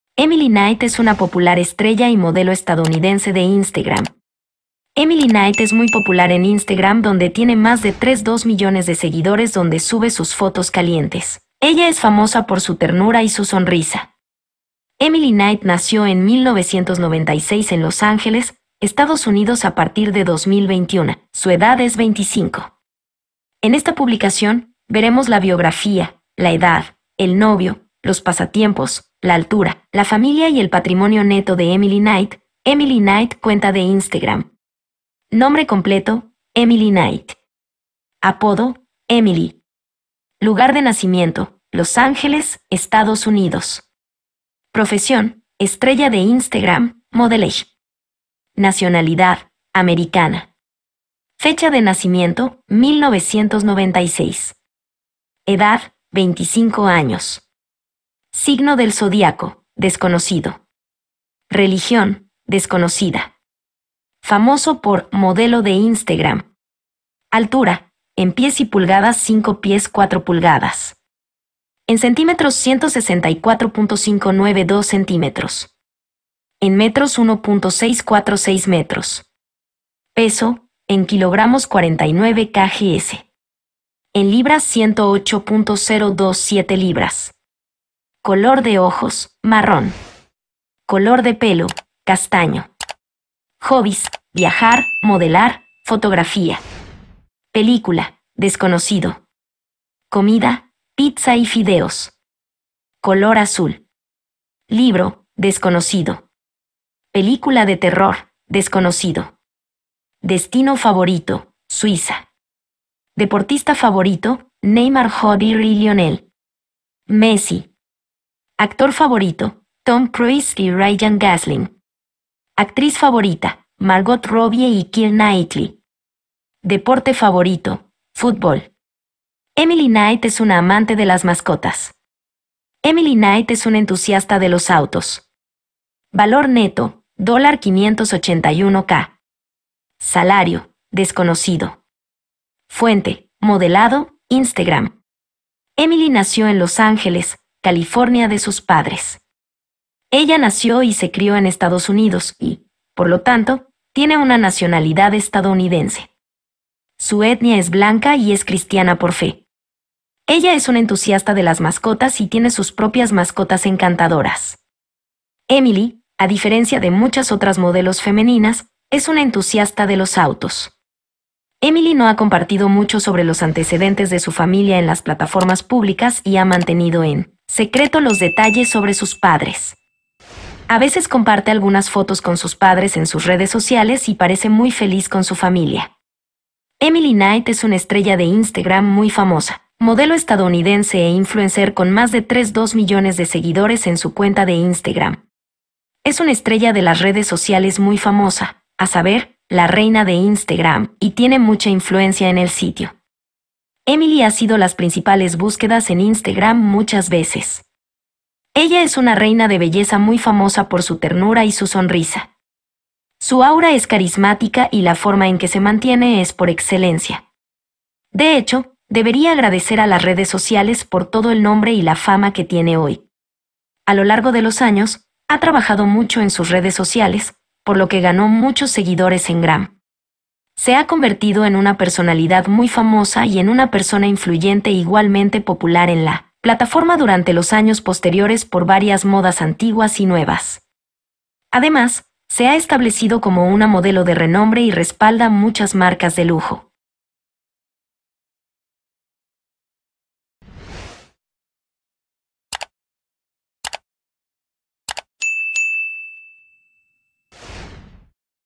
Emily knight Wiki, Biography, Age, Family, Career, Facts
Emily nació en Los Ángeles, California de sus padres. Ella nació y se crió en Estados Unidos y, por lo tanto, tiene una nacionalidad estadounidense. Su etnia es blanca y es cristiana por fe. Ella es una entusiasta de las mascotas y tiene sus propias mascotas encantadoras. Emily, a diferencia de muchas otras modelos femeninas, es una entusiasta de los autos